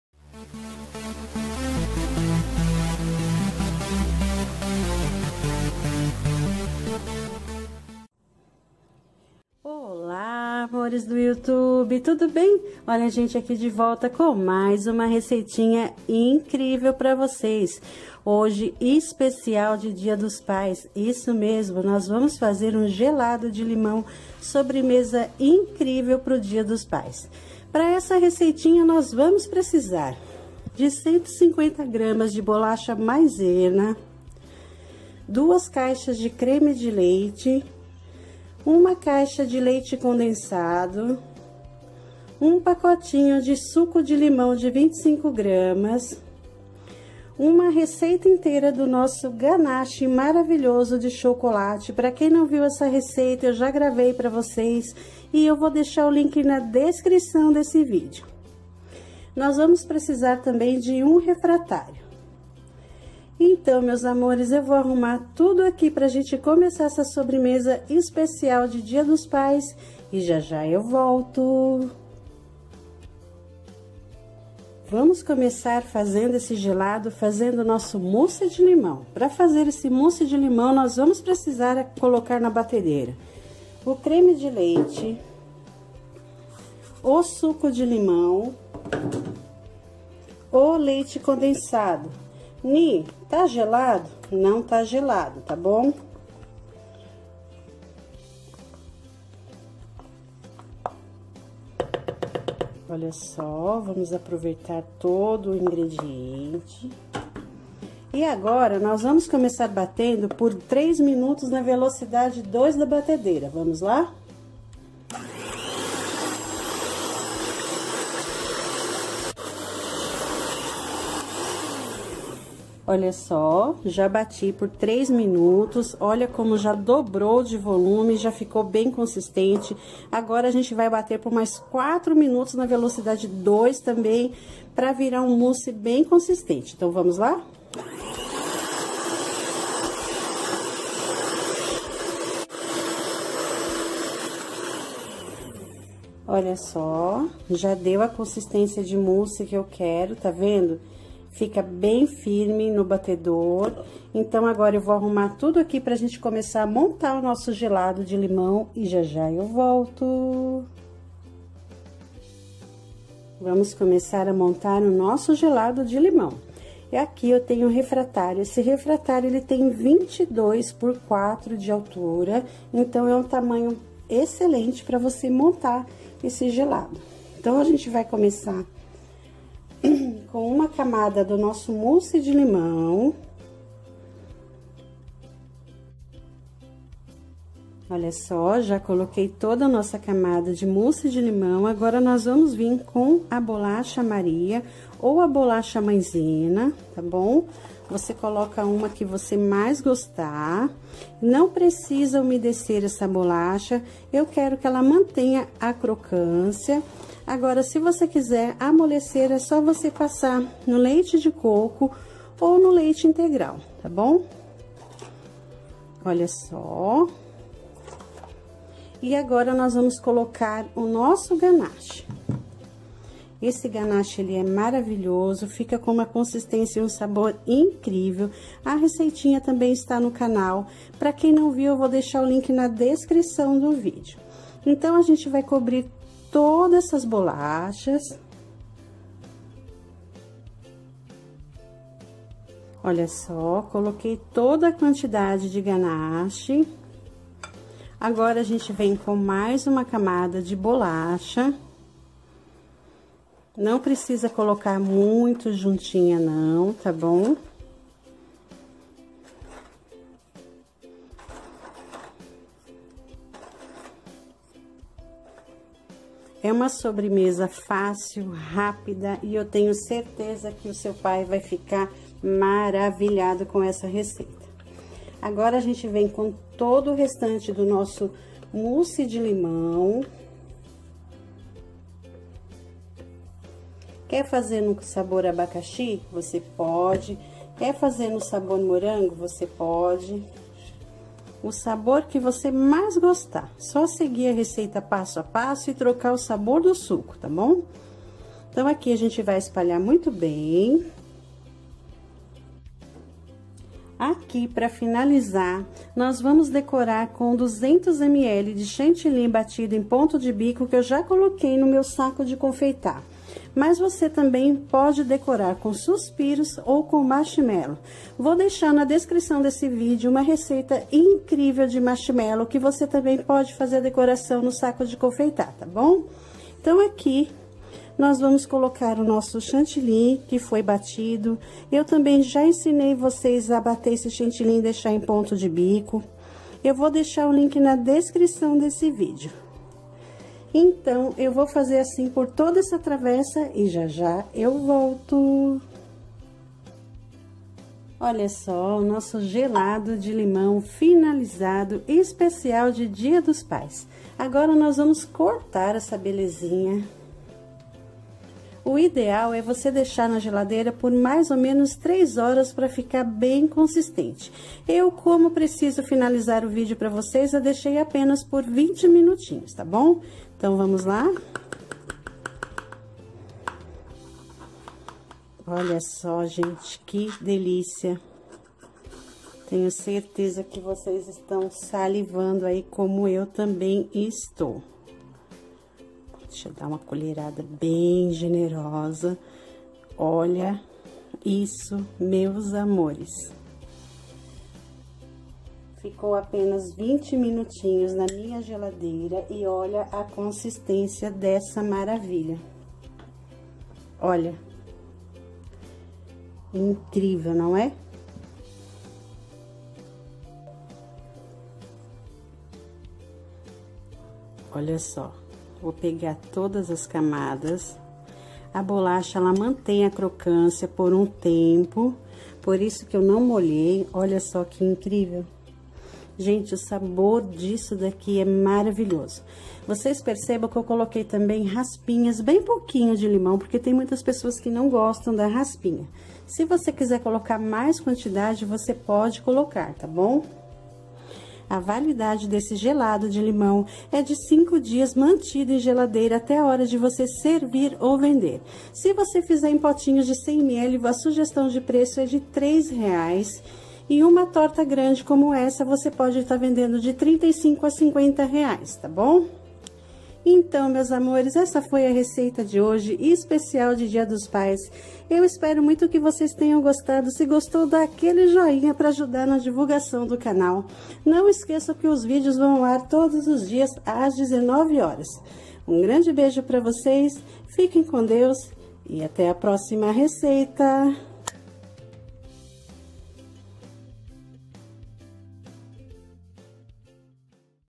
SOBREMESA GELADA DE LIMÃO ESPECIAL DE DIA DOS PAIS.
Obrigada a todos pelo carinho bjs 😘😘

ENDEREÇO POSTAL 🎁🎁⬇️
Onicia Alves
Caixa postal N°13
Itaquaquecetuba-SP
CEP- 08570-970

🔹Fan Page ➡️ Nicia Trufas & Balas Geladas

Ingredientes ⬇️
Uma caixa de leite condensado (395)g✔️
150g de bolacha maisena ✔️
Duas caixas de creme de leite (400)g✔️
Um pacote de suco de limão de 25g ✔️
Uma receita do ganache link do vídeo acima 👆✔️
Validade é de 5 dias ✔️
Sugestões de preços 💲
Pote grande  35 a 50 reais ✔️
Potinhos de 100ml 3,00 a 3,50 ✔️
Modo de fazer ✔️ Vamos ver o vídeo 😘😘